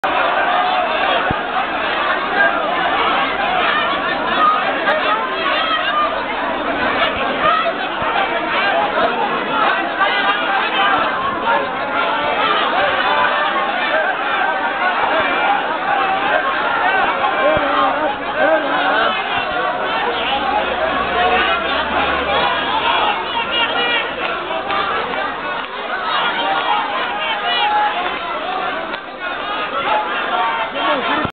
Kefaya anti-Mubarak demo كفاية تتظاهر ضد مبارك
Clashes between activists and police. Protestors chanting against Mubarak, during Kefaya's anti-Mubarak's authoritarian constitutional amendments.